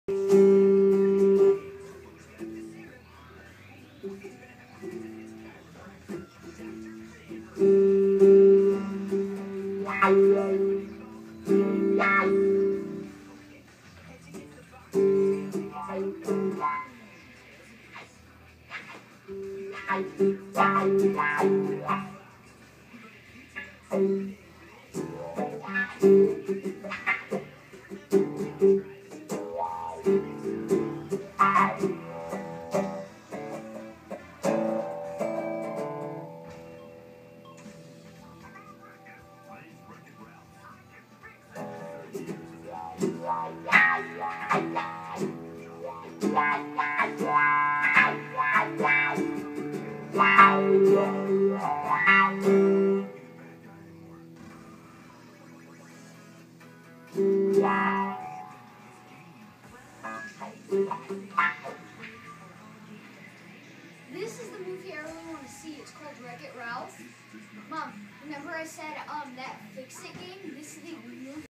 7yr old electric guitar. 1st time
Cale learning to play, decides to grab Dad's whammy etc & see what happens